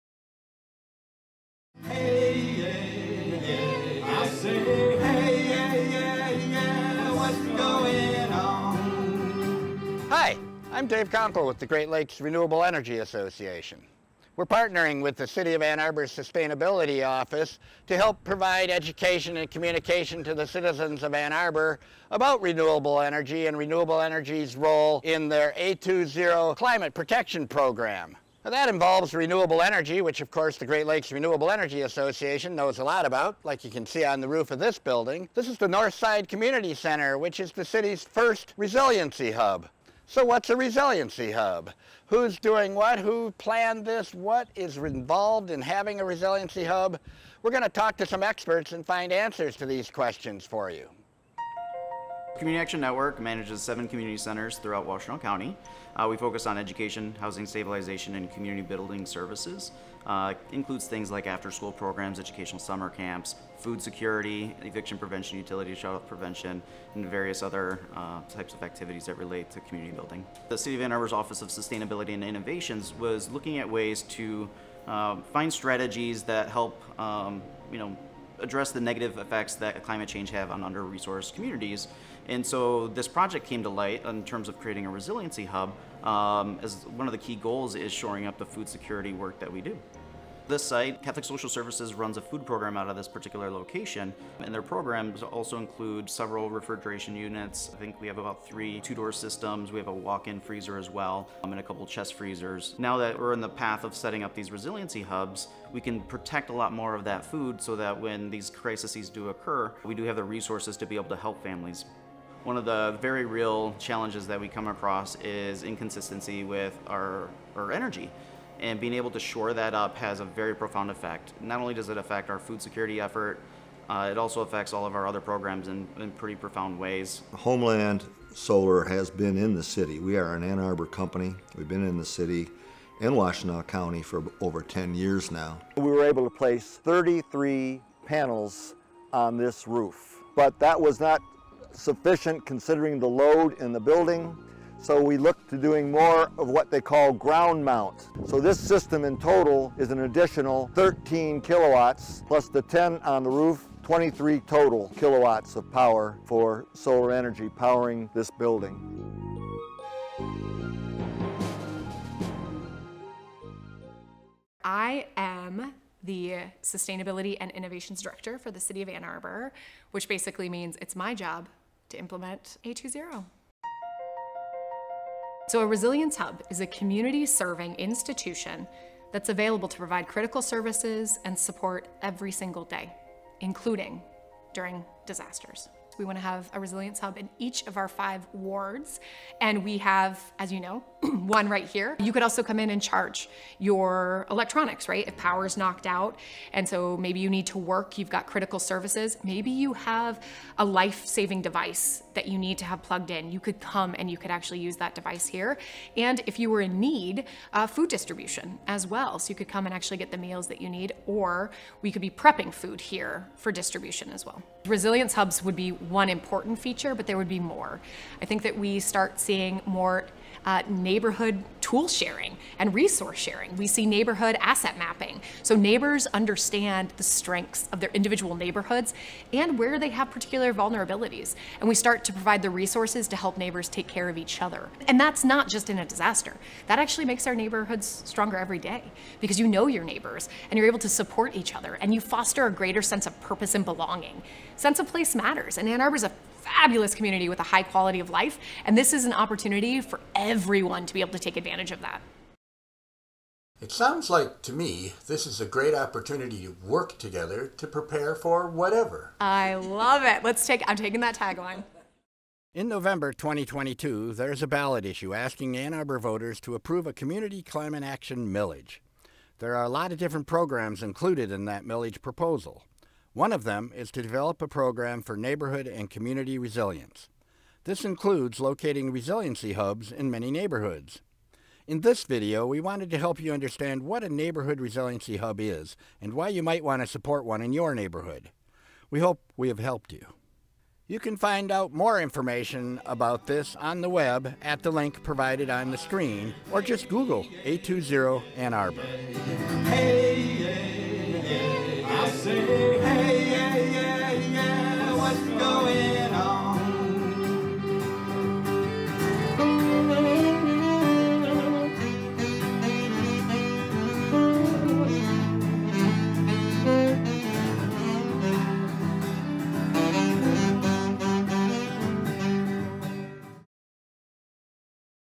What is a Resiliency Hub?
Resiliency Hubs are community-serving facilities augmented to support residents and coordinate resource distribution and services before, during, or after a natural hazard event.  Being partnered with Great Lakes Renewable Energy Association, the City of Ann Arbor's Office of Sustainability and Innovations operates Resilience Hubs at the nexus of climate mitigation, climate adaptation, and equity. They also strive to enhance community sustainability and resilience through a bottom-up approach centered on co-development and local leadership.  In this program hosted by Dave Konkle of Great Lakes renewable Energy Association, experts including Derrick Miller of Community Action Network, Larry Kerber of Homeland Solar and Missy Stults Ph.D of City of Ann Arbor discuss what Resiliency Hubs are and how they serve for the community.